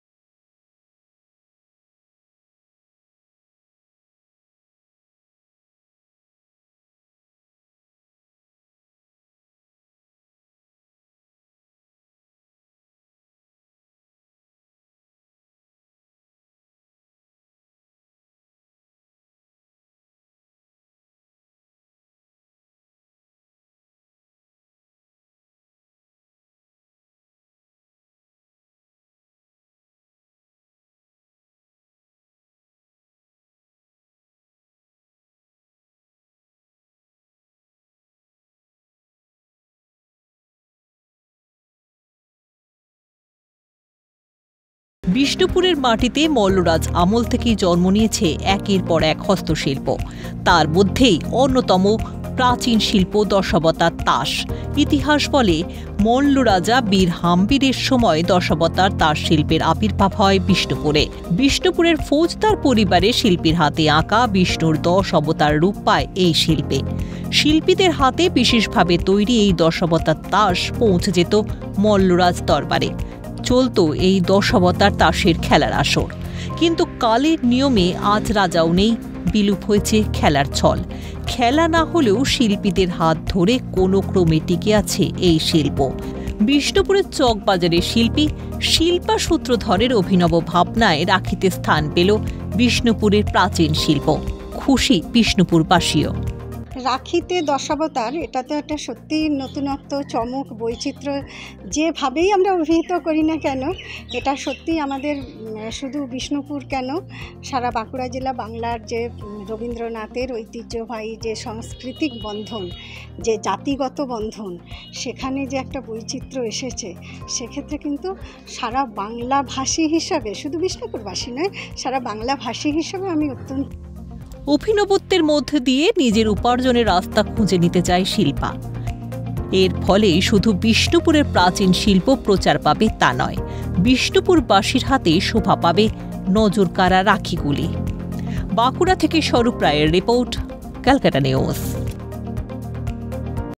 রাখীতে শোভা পেল 'দশাবতার', অভিনব চমক বিষ্ণুপুরের শিল্পীর। ক্যালকাটা নিউজ
রাখীতে শোভা পেল 'দশাবতার', অভিনব চমক বিষ্ণুপুরের শিল্পীর

রাত পোহালেই রাখী উৎসব।ব্যস্ততা তুঙ্গে। এবার রাখীতে অভিনব ভাবনা বিষ্ণুপুরের চকবাজারের শিল্পীর।দশাবতার রূপ পেয়েছে রাখীতে। হারিয়ে যাওয়া ইতিহাসকে নতুন ভাবে জিইয়ে রাখার চেষ্টা বিষ্ণুপুরের চকবাজারের শিল্পী শিল্পা সূত্রধরের।